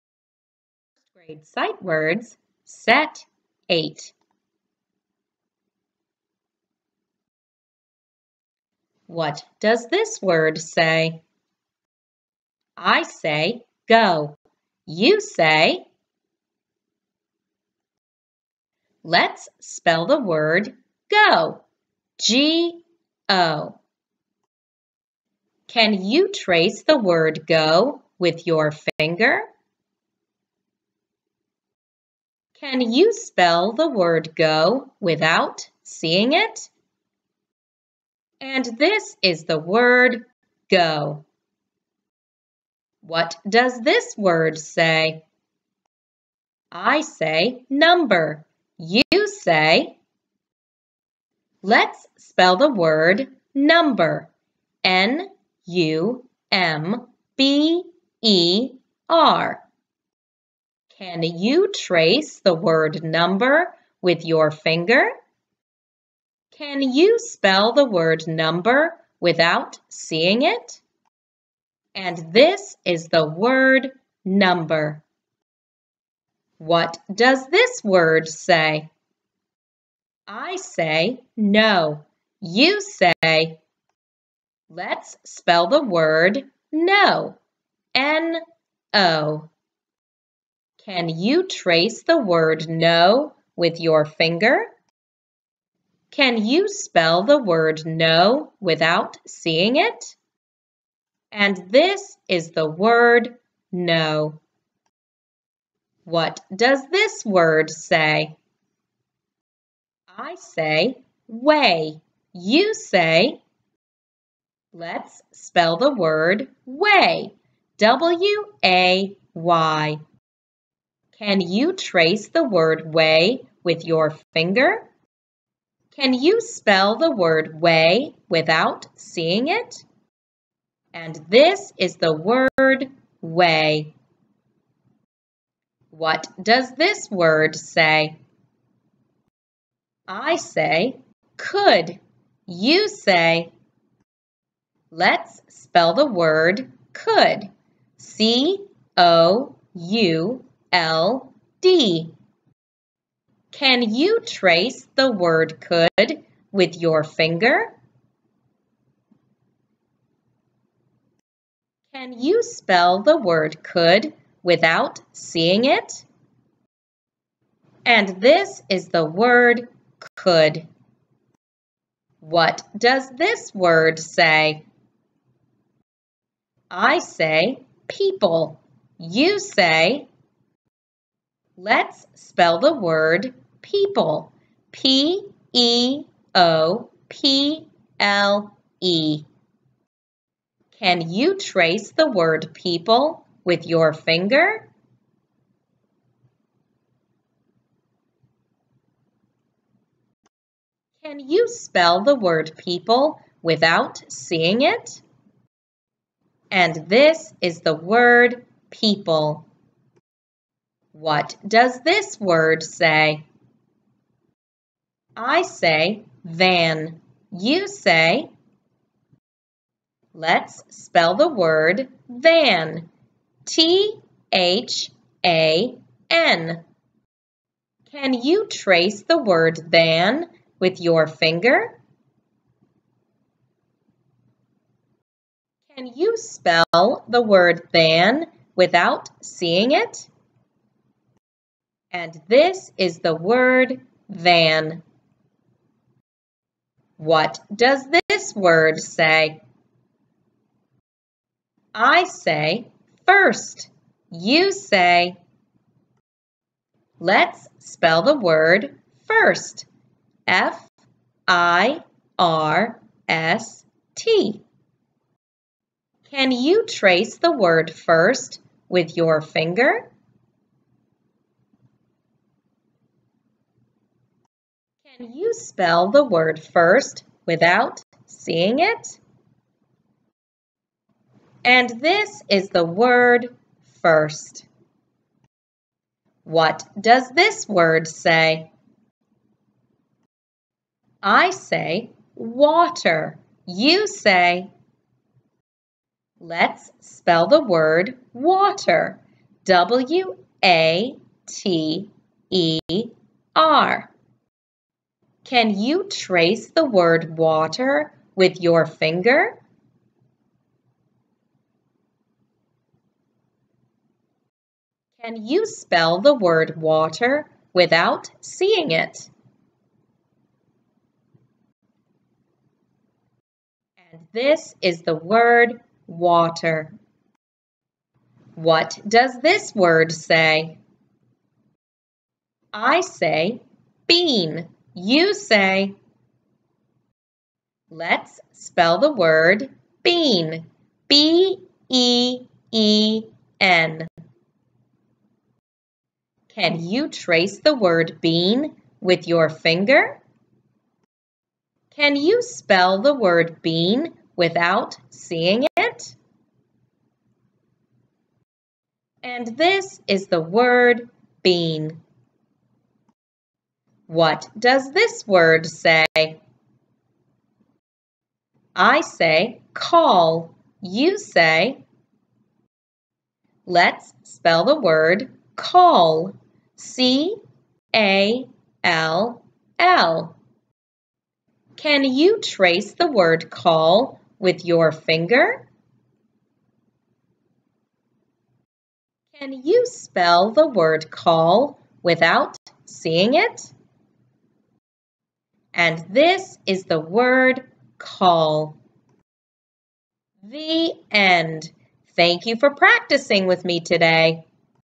First Grade Fry Words Sight Word Movie 3 (23- 33)
If your child has mastered reading and writing all 57 kindergarten Fry sight words, this video helps students to independently practice the following sight words:  go, number, no, way, could, people, than, first, water, been, & call.